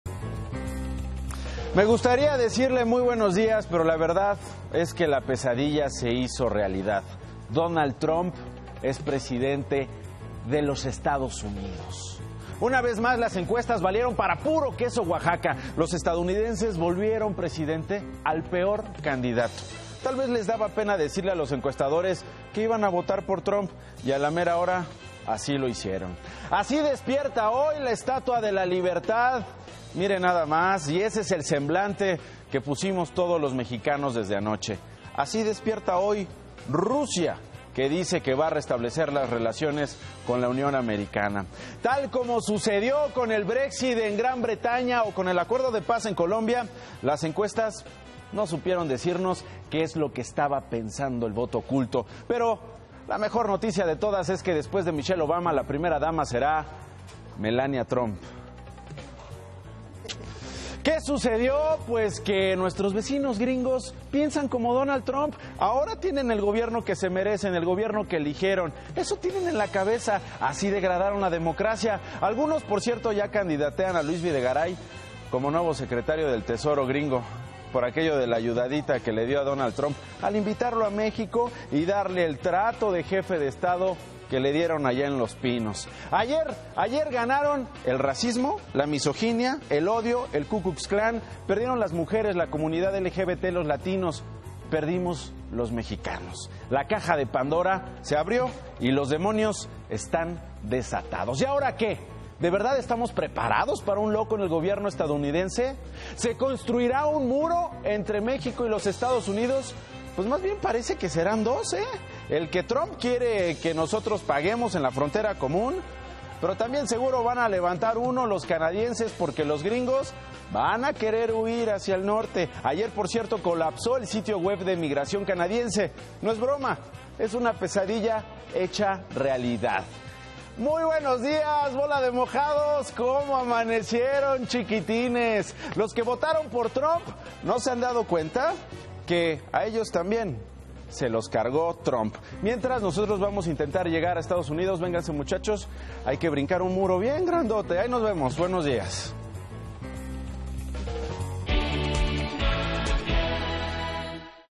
Las encuestas valieron puro queso Oaxaca
Contra todo pronóstico, los estadounidenses decidieron tener un presidente racista y misóngino. ¡A construir el muro!